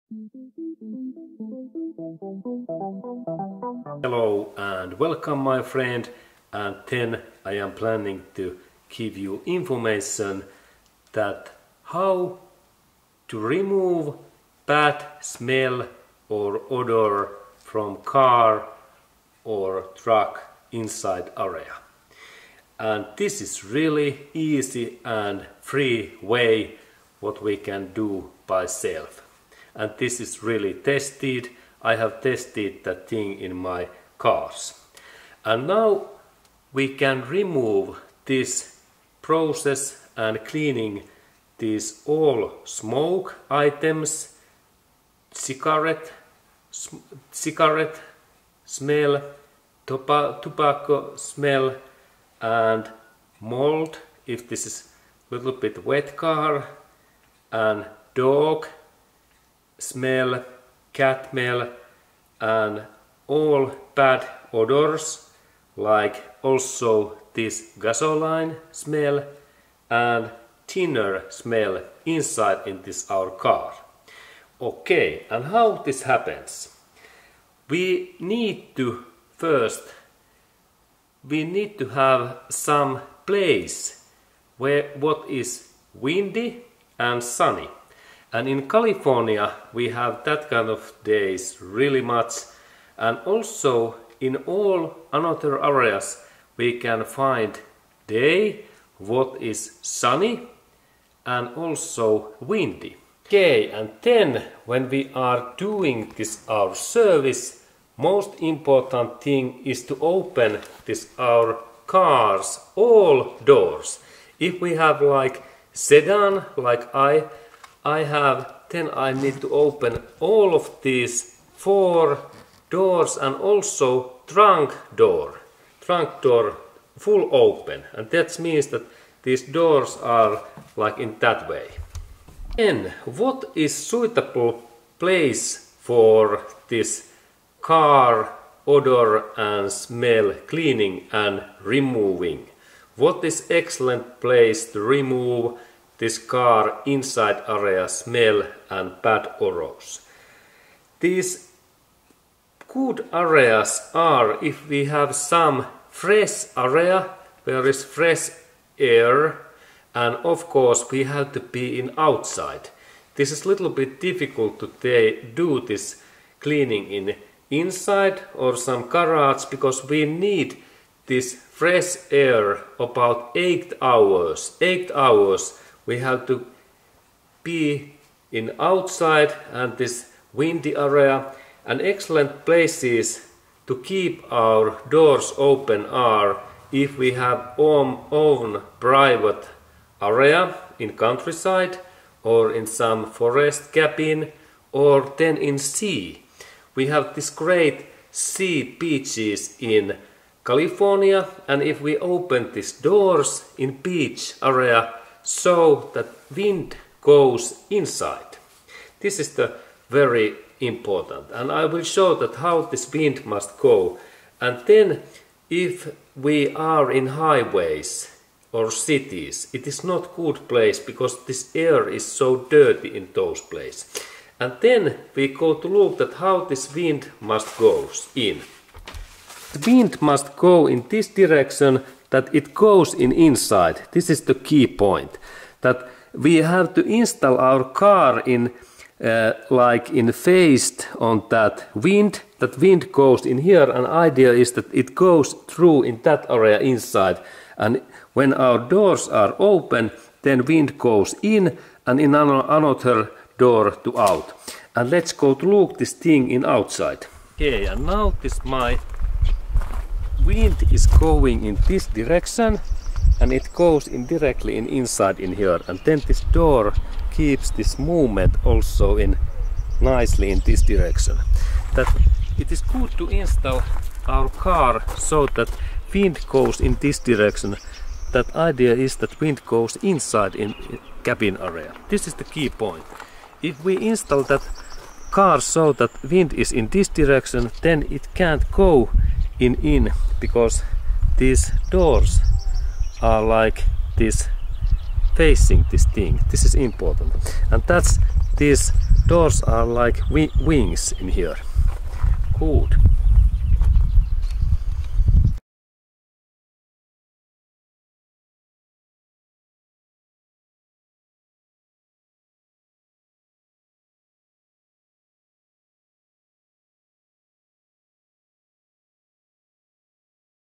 Easy and FREE! How to remove BAD ODOR or SMELL away from inside area car or truck? Look Info
¡Fácil y GRATIS! ¿Cómo eliminar el MAL OLOR o el OLOR del interior del coche o camión? Ver información
Elimina el mal olor del tabaco, cigarro, humo, perro, gato, gasolina, disolvente
Remove bad odor from tobacco, cigar, smoke, dog, cat, gasoline, thinner, odor. Keep doors Open 8 Hours ! And Repeat 3 times !

Sections in the video:
0:00​ Introduction
0:40​ Smoke, thinner, dog smell
1:24 Place
1:47 Doors Open 8 Hours ! Repeat 3 times !
3:44 Wind movement